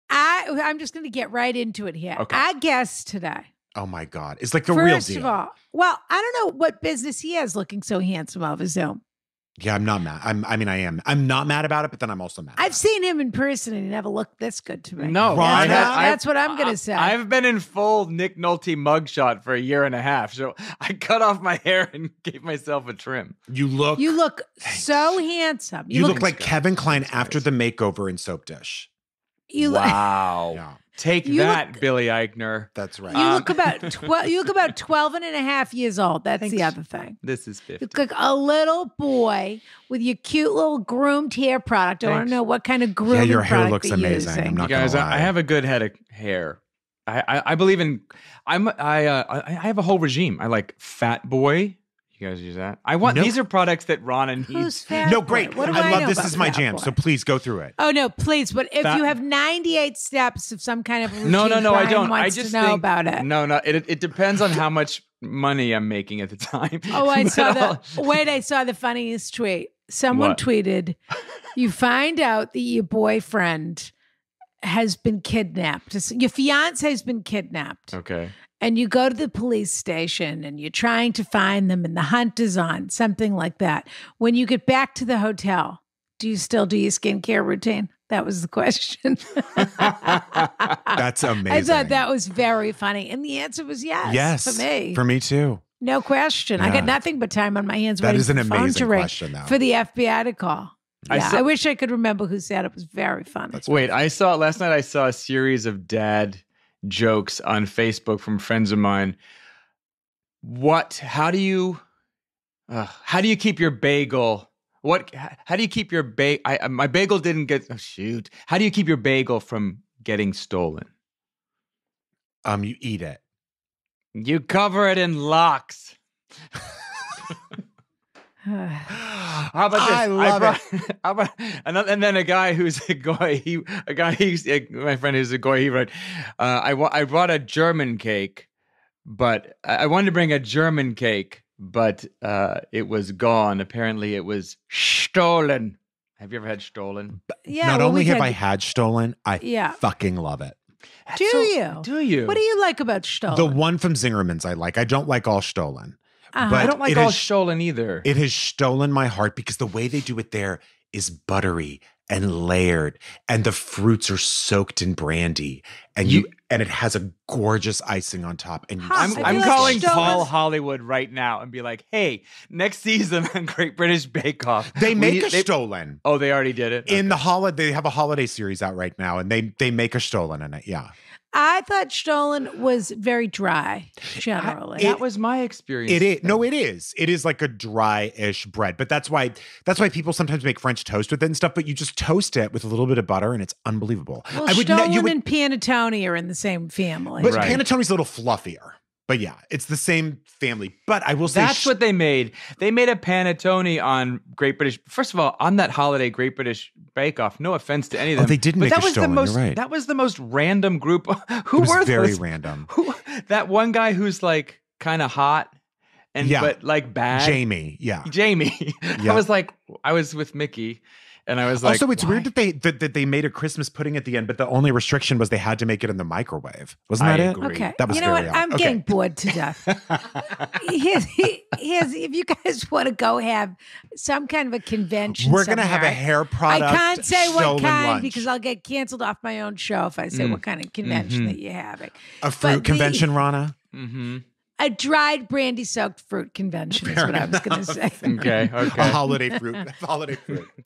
Free Feed 5 with Jason Butler Harner
In this week's Free Feed 5, we marvel at our special guest Jason Butler Harner's ability to look as a fabulous as he does, and we chat about dad jokes and Great British Bake Off.

For more be sure to catch us wherever you get your podcasts!